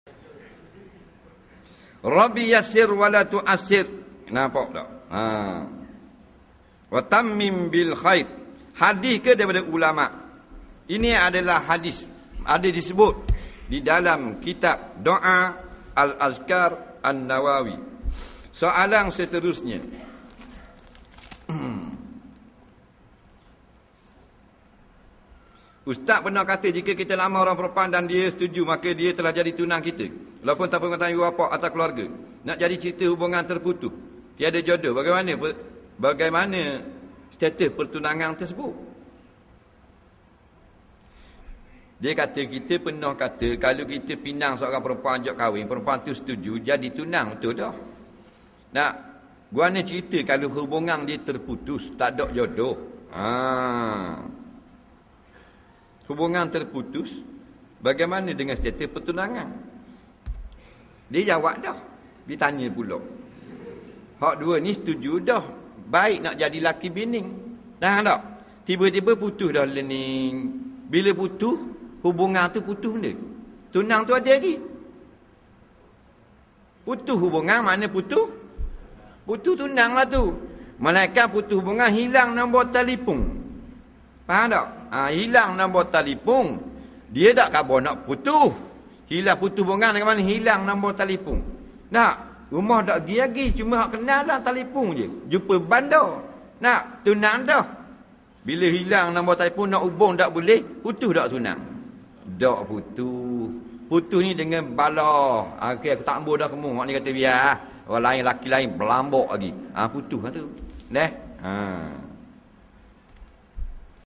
Ust Azhar Idrus- ' Robbi Yassir '| Bertunang & Putus
[Pembatal Wuduk] Robbi yassir wala tu 'assir, watammim bil khoir ;
(Ya Allah! permudahkanlah segala urusan, janganlah Engkau persulitkannya, sempurnakanlah dengan kebaikan)